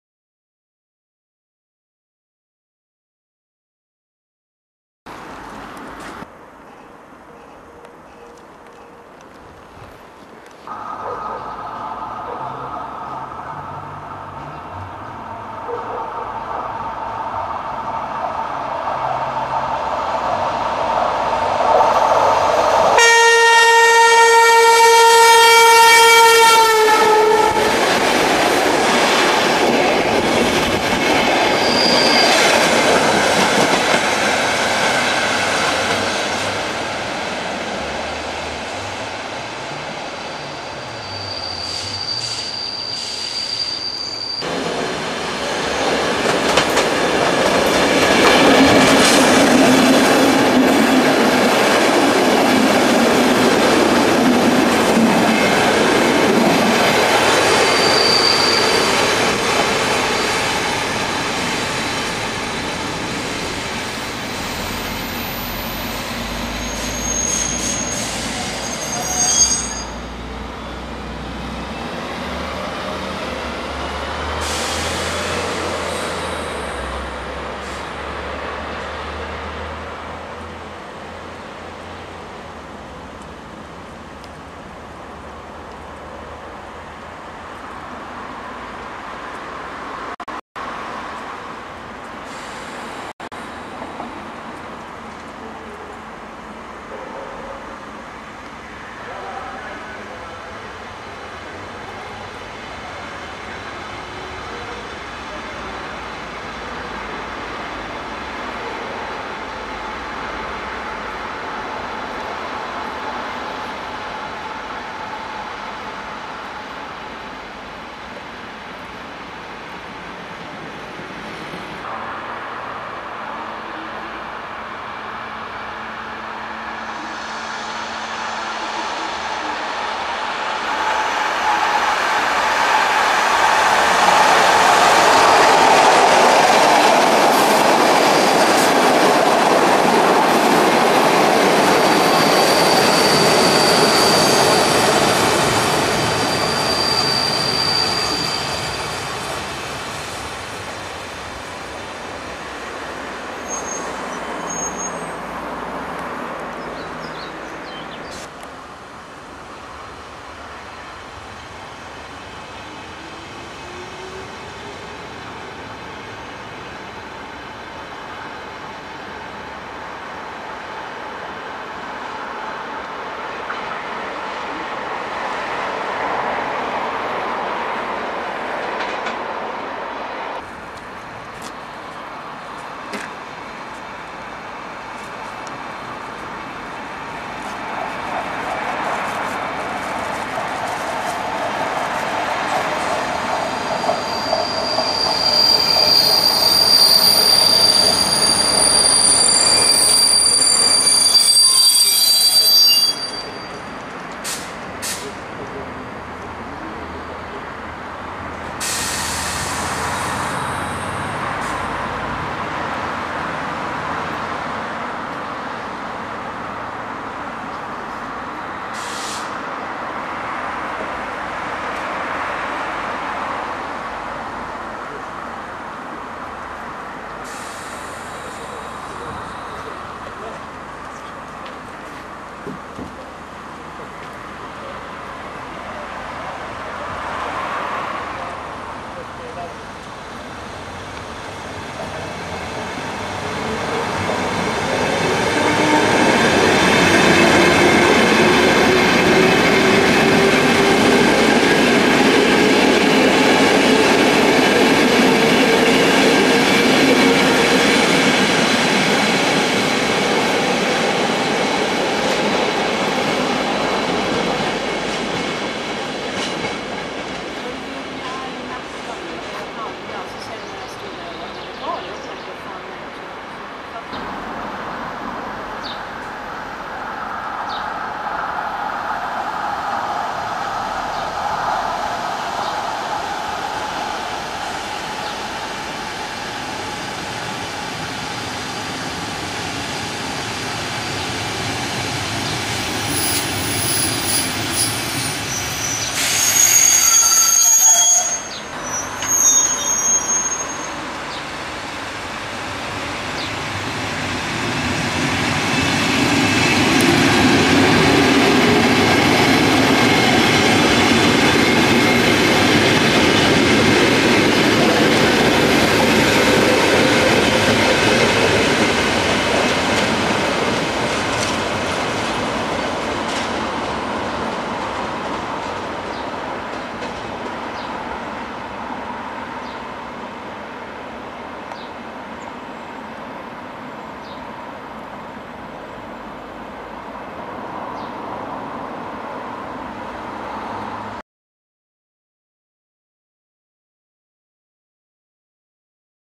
Station NACKA Saltsjoebahn
Aufnahmen am Bahnhof NACKA der Saltsjoebahn Stockholm gemacht